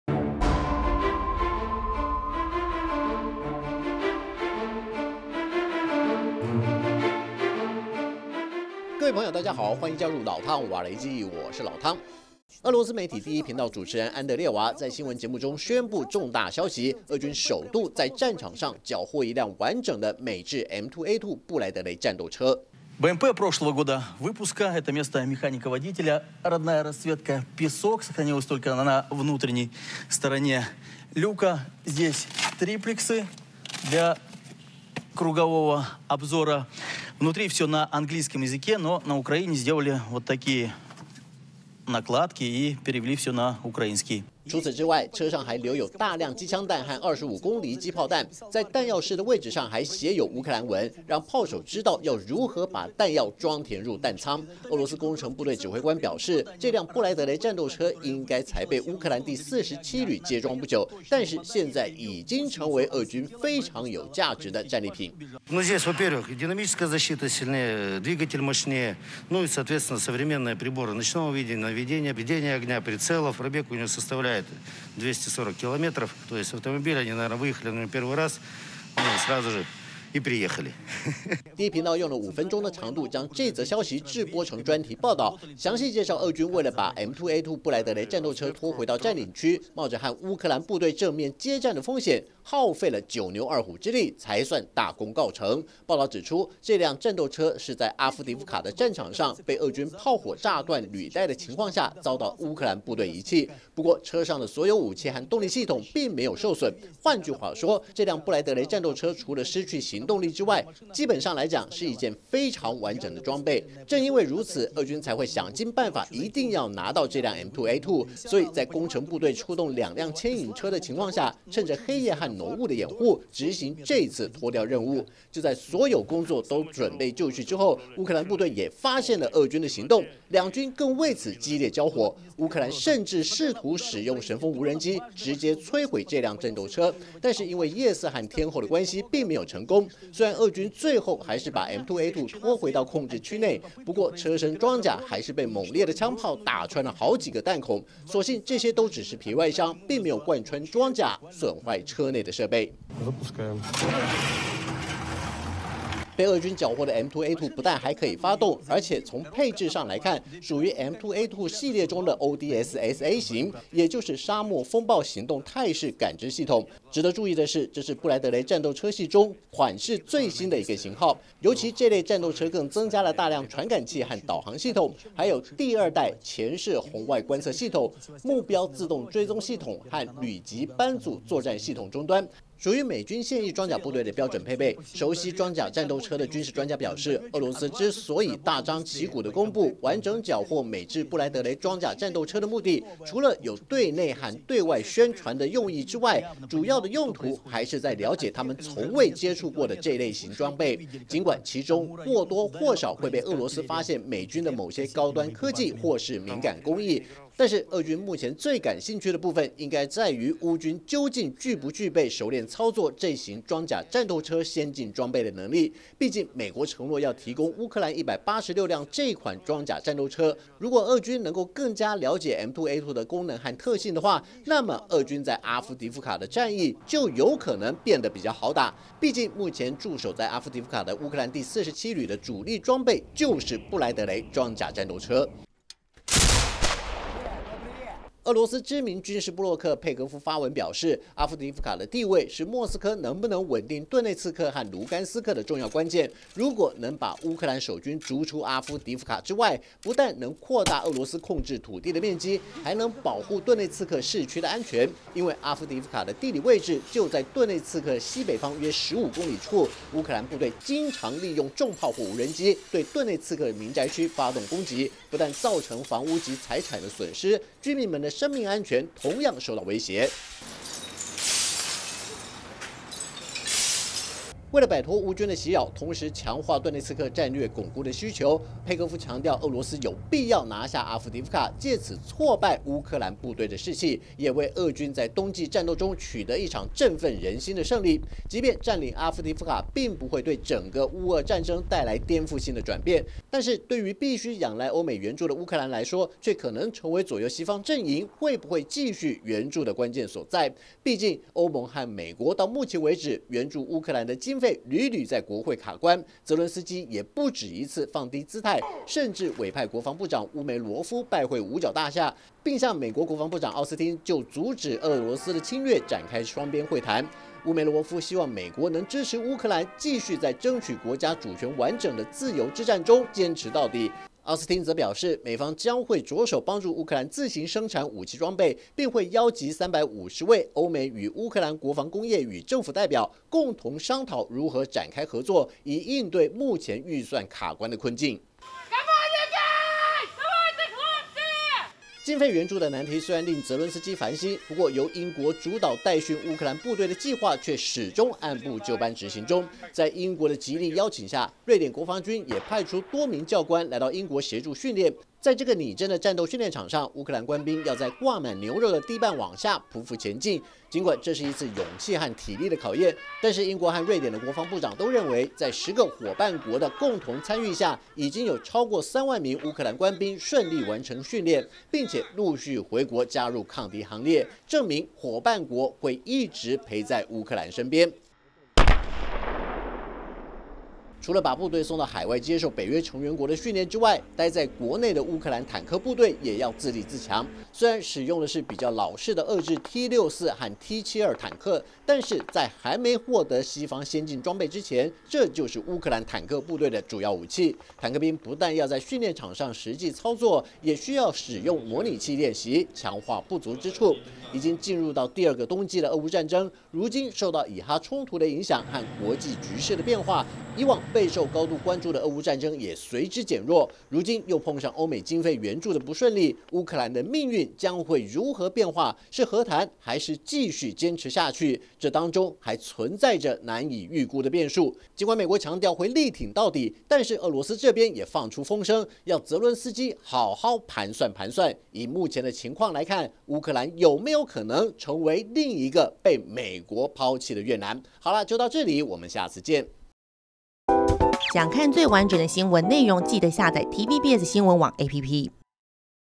M2戰鬥車成俄軍戰利品 援助卡關澤倫斯基很煩心｜老湯話你知Podcast#95｜TVBS新聞
俄羅斯媒體報導，在阿夫迪夫卡繳獲一輛堪稱完整的美製「布萊德雷」戰鬥車，也是俄軍首度在烏克蘭戰場上，擄獲美軍的現役裝備。